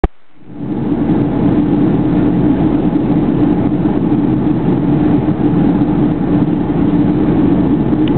leonardo reis voando pela gol 2005 03/04
Maestro Leonardo Reis de Monte alegre de Minas voando pela Gol de Belo Horizonte para uberlândia em 2005 pousando em uberlândia